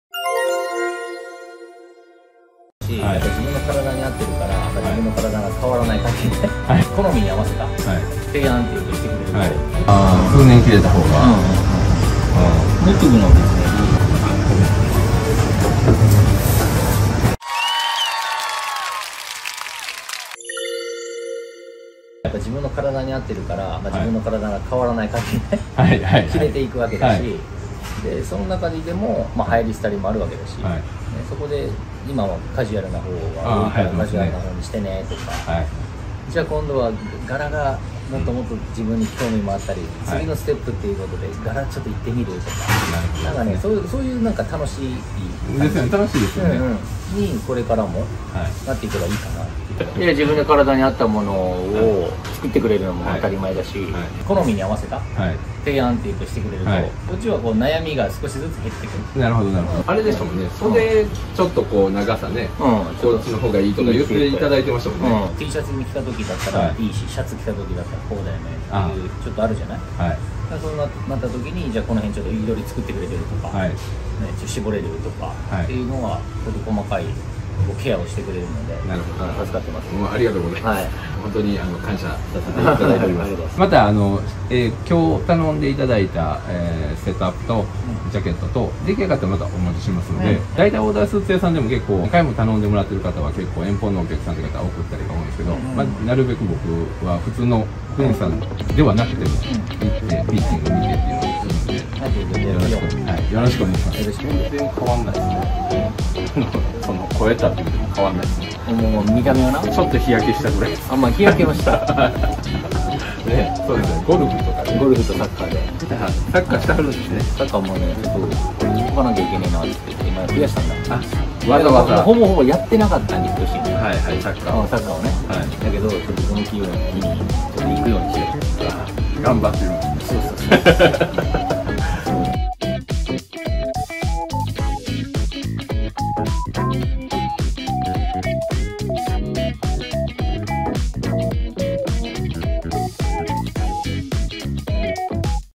[ゲスト企画]サッカー元日本代表 福西 崇史リラックススーツをオーダーメイドする③
元サッカー日本代表 福西さんとのオーダースーツ選び
シリーズ3本目の動画

動画再生時間は短いですが、楽しい雑談も少し入ってるので是非ご視聴ください

福西 崇史さんのYouTubeチャンネルも是非